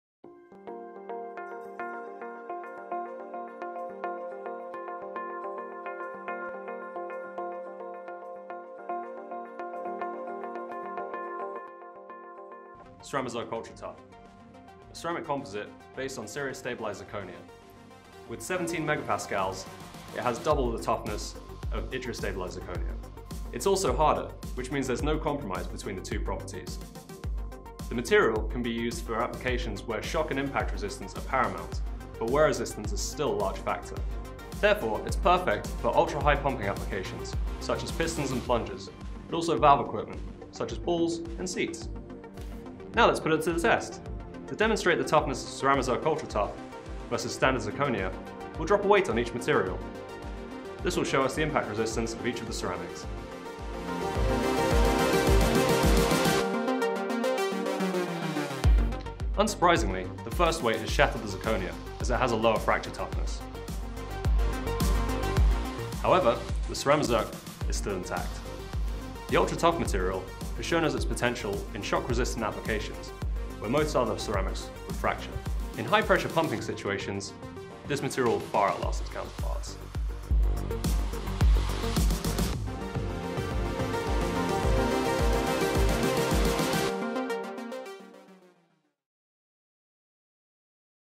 Advanced High Fracture Toughness Zirconia Composite - CeramaZirc Ultra Tough
Dubbed ‘ceramic steel’ Zirconia (ZrO2) ceramic materials offer a combination of high hardness, wear and corrosion resistance while still maintaining one of the highest figures for fracture toughness amongst ceramic materials. It has remarkably high fracture toughness and impact resistance while maintaining above average values for bending strength and hardness.

In this video we put it's impact resistance to the test.